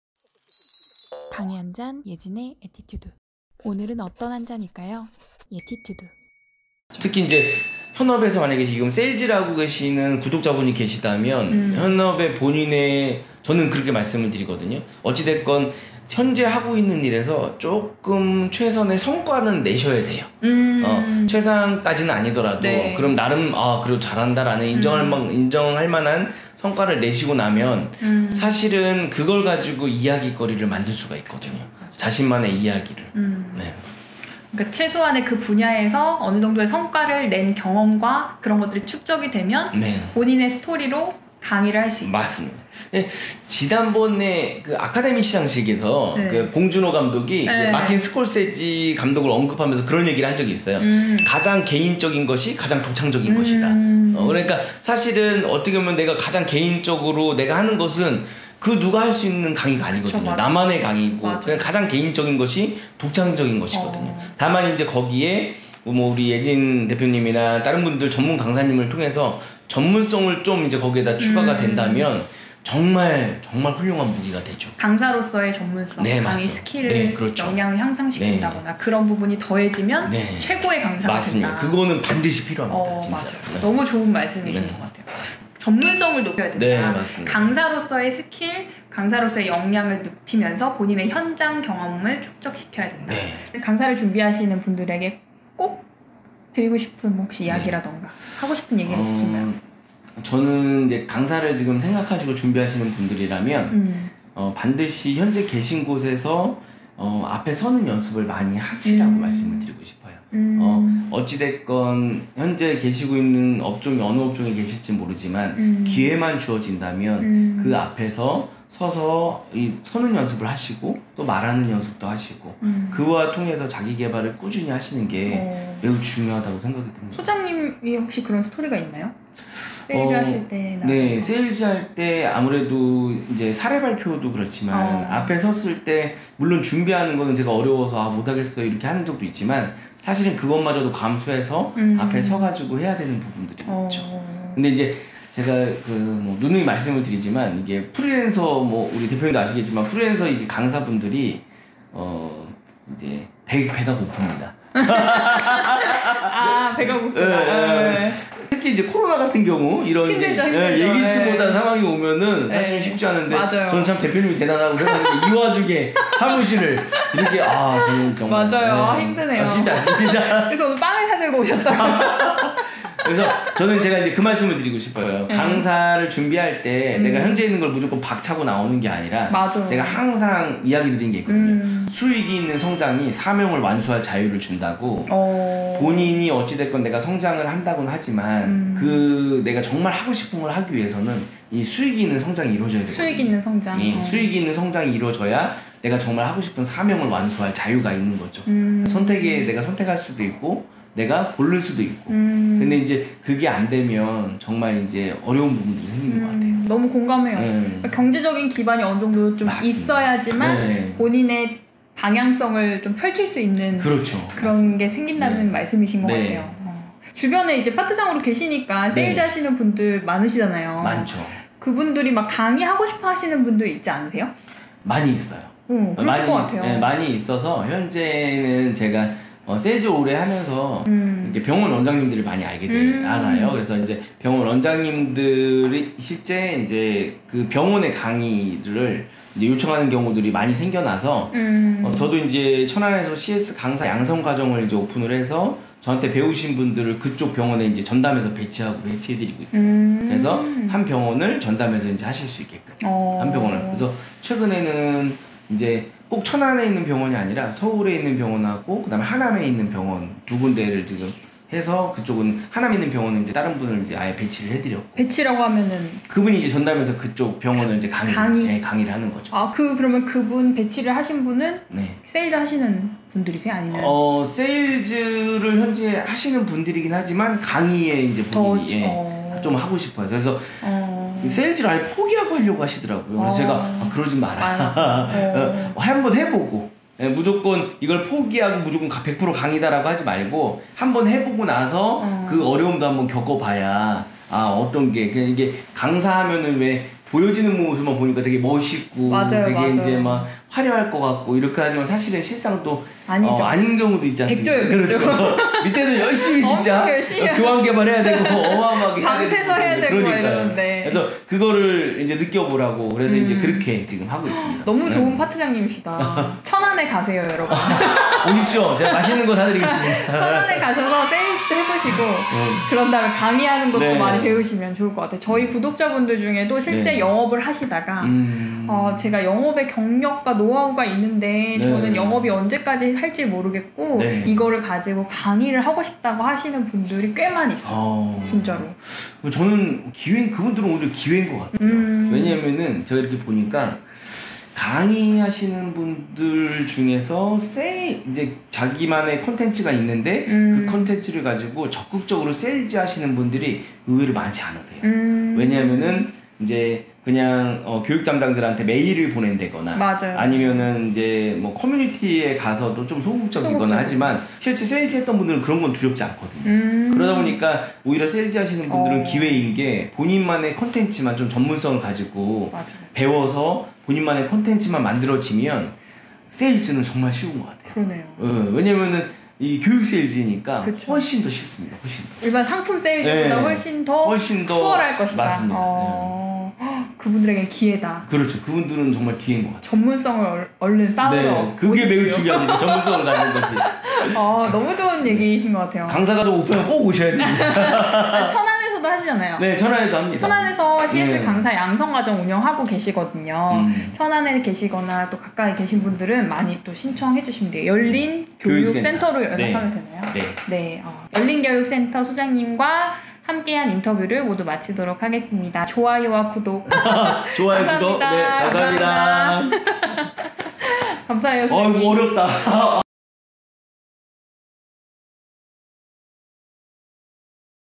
세일즈강사 CS강사되는법 영업하다가 강의를 하고싶어졌어요 강의한잔
안녕하세요! 예티튜드 손강사입니다~
오늘의 강의한잔은 영업, 세일즈를 하시다가 강사를 하고싶어하시는 분들에게
도움이 될 영상을 준비했습니다!
영업하시면서 세일즈강사, CS강사 하고싶어하시는 분들이
연락을 종종 주시는데요~
영업경력을 어떻게 쌓고 나오면 좋을지!
영업경력이 나중에 강사에게 어떤 도움이 되는지!
강사를 준비하면서 반드시 명심해야 될 것!
풍부한 영상을 준비했습니다~
궁금한 게 있으시면 언제든지 문의주세요~
여러분과 자주 소통하고 싶은 손강사 입니다^^